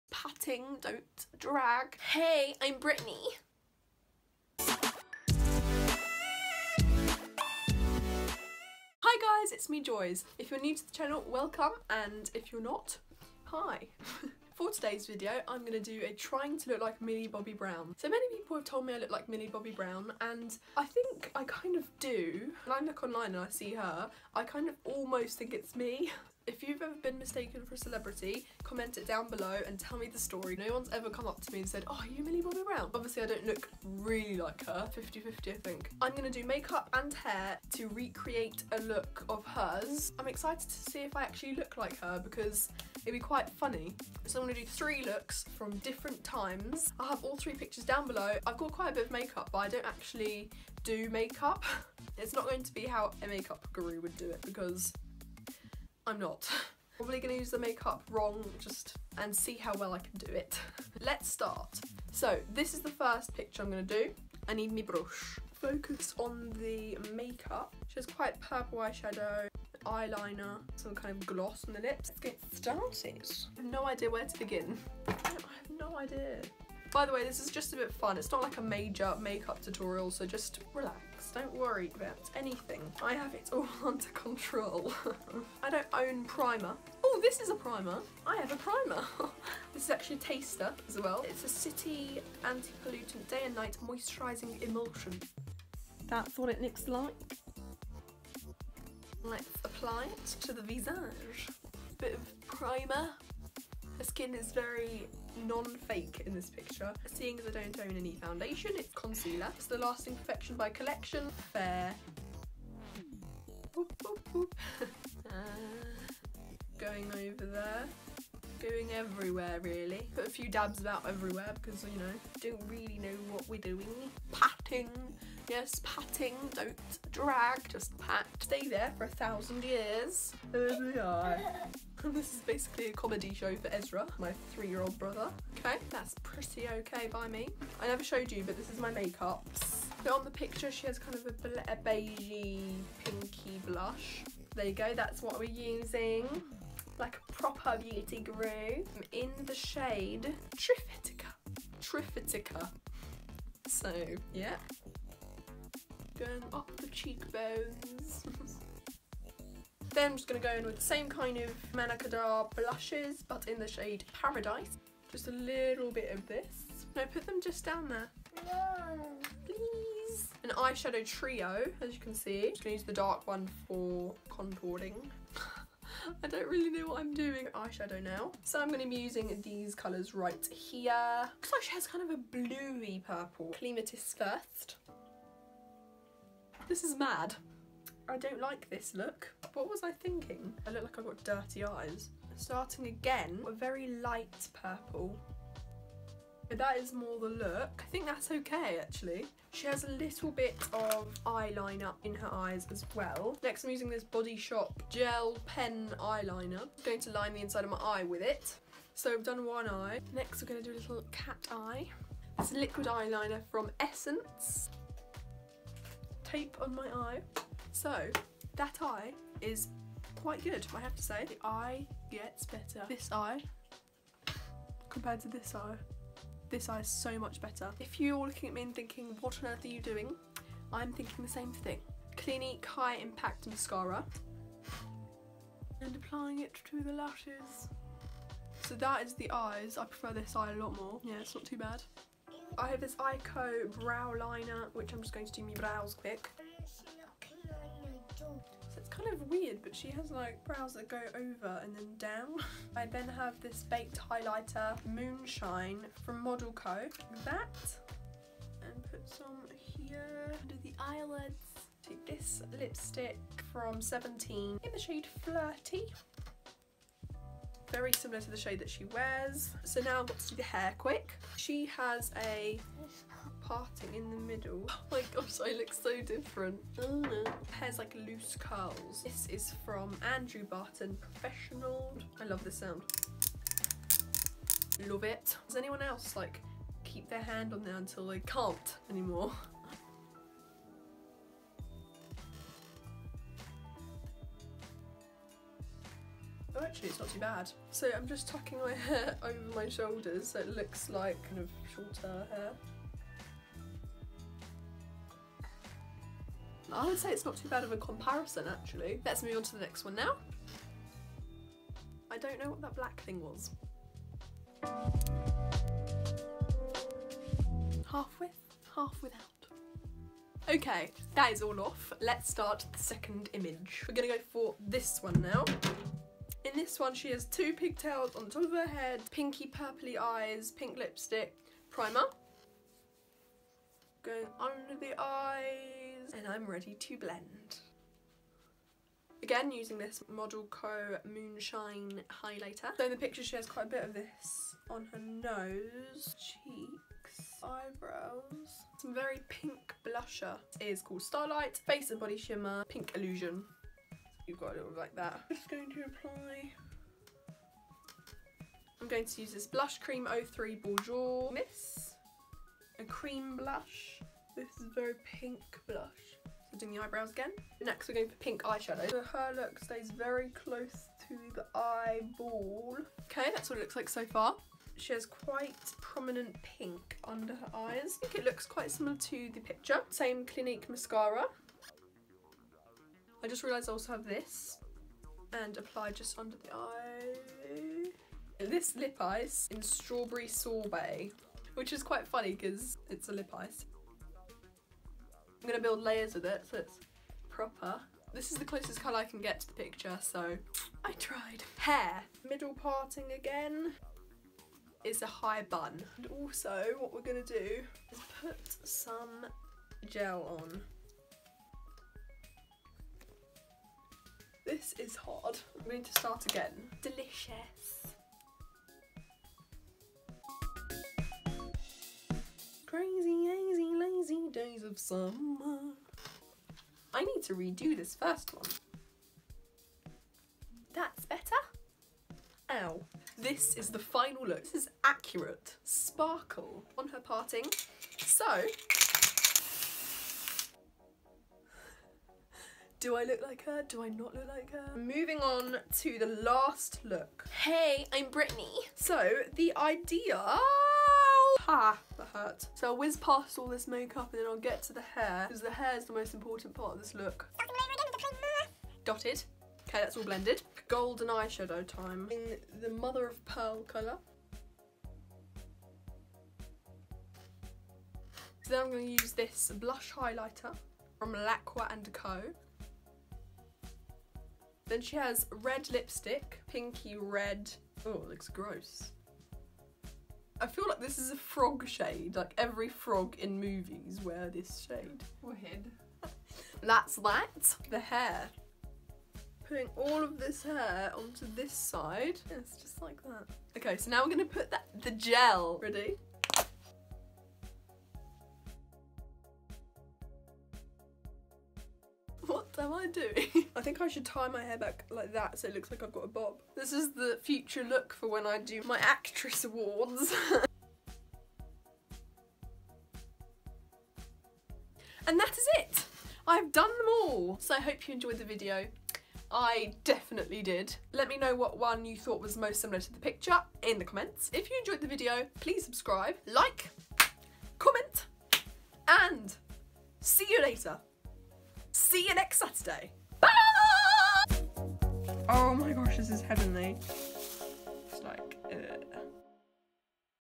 TRYING TO LOOK LIKE MILLIE BOBBY BROWN?!? | ItsMeJoyz | Makeup
TRYING TO LOOK LIKE MILLIE BOBBY BROWN?!? | ItsMeJoyz | Makeup and Hair Lookalike

Hello Fellow People!
I hope you enjoyed this Saturdays video, I certainly enjoyed the filming of it :))) Comment which out of the 3 looks you think is most similar to the picture.